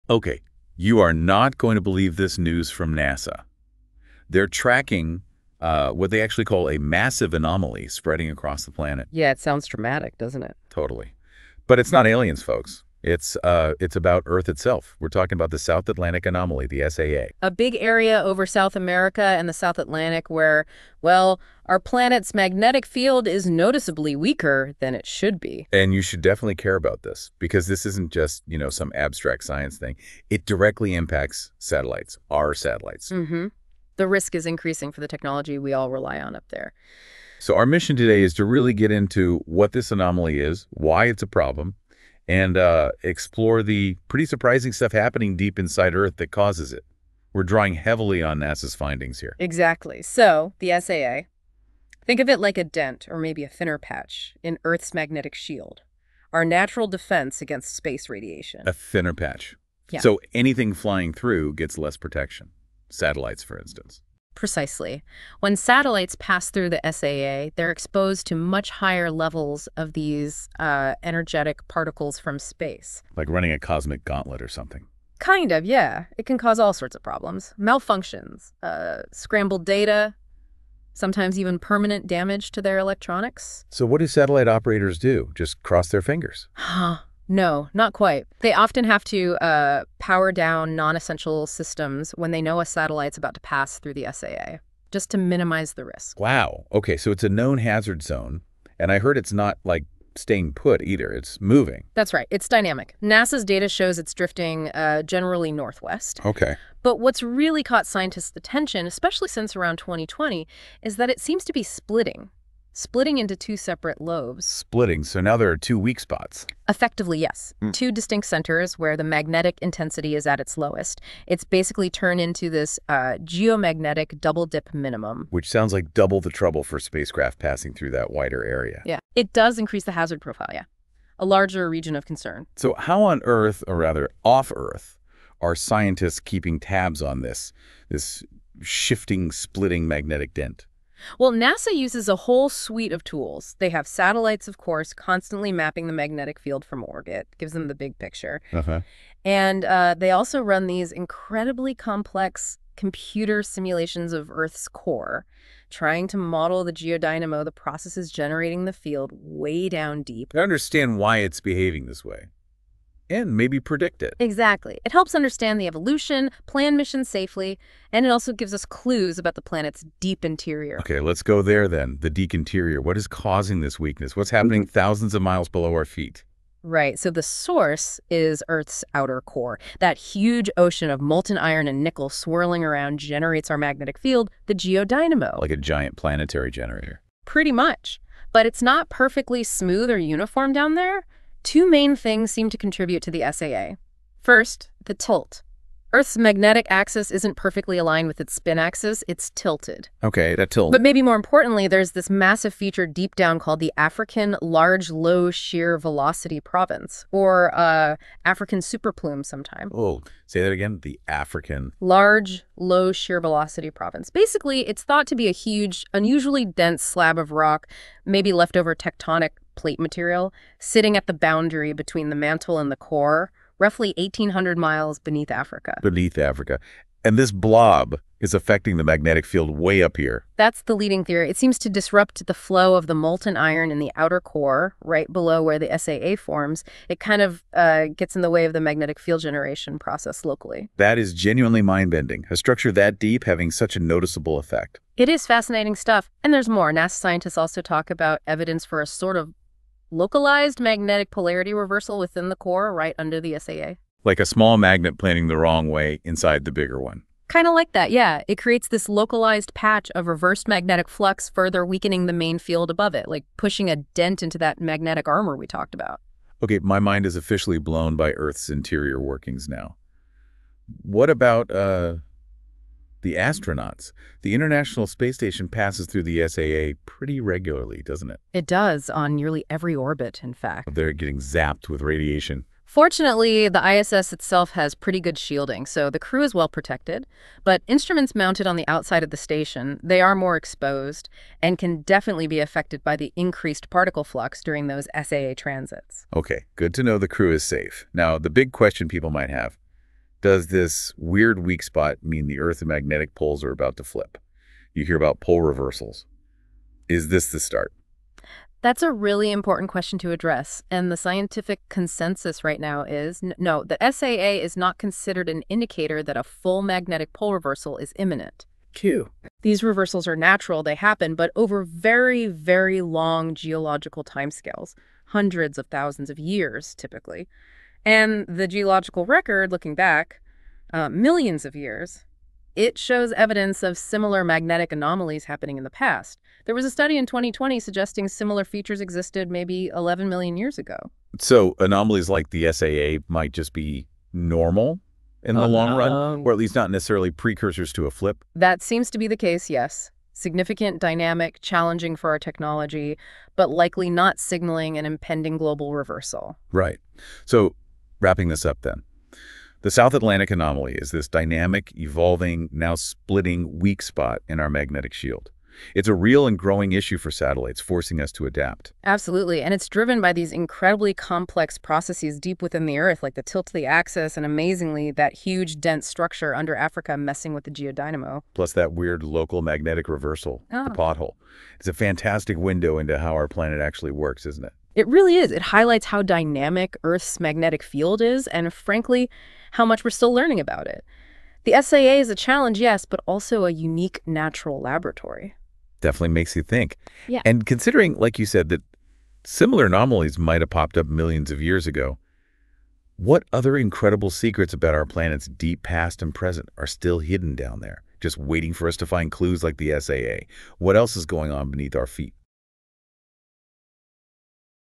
The Deadly South Atlantic Anomaly Can Do This To Earth's Satellites | NASA Sounds Alarm
This video explains the South Atlantic Anomaly (SAA), a region where Earth's magnetic field is weaker, posing risks to satellites and the ISS due to increased exposure to high-energy particles. The video details how the SAA is monitored by NASA, describing its location over South America and the South Atlantic Ocean. It highlights the anomaly's dynamic nature, mentioning its drift northwest, expansion, and split into two lobes. The video also discusses the SAA's origins, linking it to processes in Earth's core and the influence of a large dense structure beneath Africa, and clarifies it's not a sign of an impending pole reversal. Finally, it touches on the preventive measures taken by satellite operators to mitigate the risks posed by the SAA.